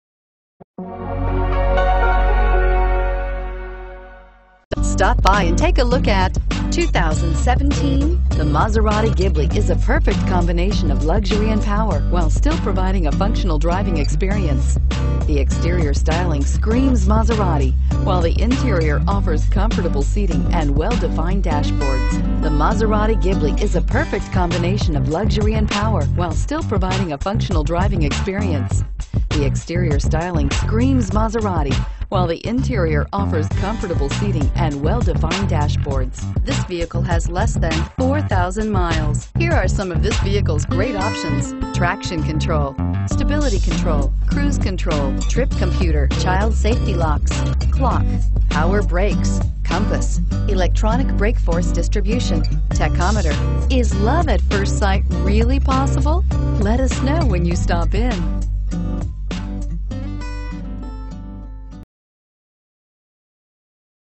2017 Maserati Ghibli Sedan for sale in Cary NC
2017 Maserati Ghibli Sedan for sale in Cary NC - Stock # M1063
Year: 2017
Engine: 3.0L V-6 cyl
Transmission: ZF 8-Speed Automatic
MPG Range: 17/24
Exterior Color: Grigio
Address:
VIN : ZAM57XSL9H1242322